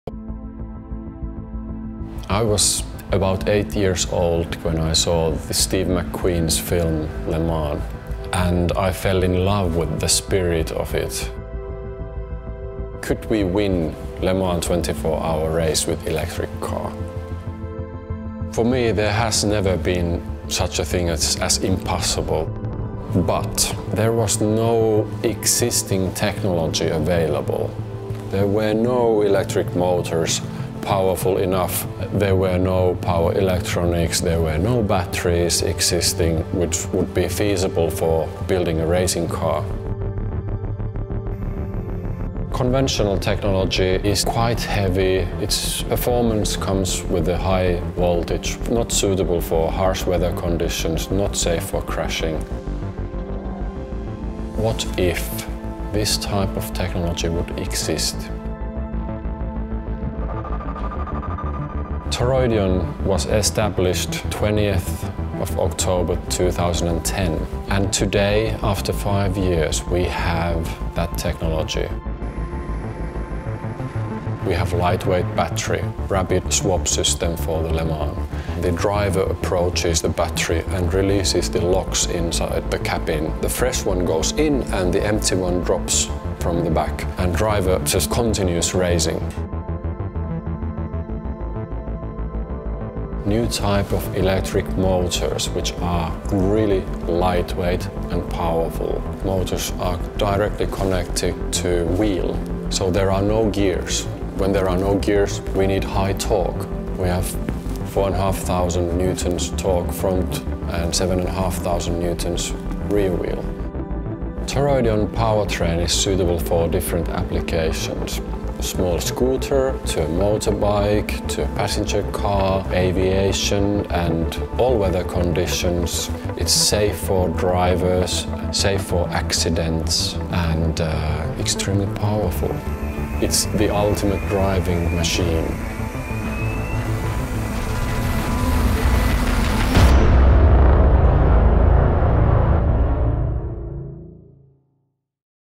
Toroidion - The Story Begins
It started with a simple question. Could an electric car win at Le Mans?

We first considered issues of safety, performance and the weight-to-power ratio of existing technologies. The required performance meant an unsafe voltage and available components were too heavy. We needed to create completely new electric motors and an innovative powertrain to drive them.

Toroidion Direct Drive Electric Motors generate massive torque even at high speed, delivered directly to each wheel without the need for gears or transmissions.

Toroidion 1MW Concept car is the first product that harnesses our patent pending powertrain.

Developed and made in Finland.

Video Production: KAUAS Creative Oy